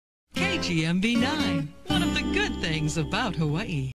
KGMB (CBS) Station ID 1995
This is KGMB's station ID from 1995
KGMB is located in Honolulu, HI